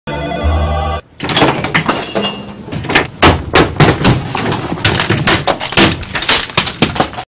Windows XP Users After April 8 2014
You need to upgrade to Windows Vista, Windows 7 or Windows 8.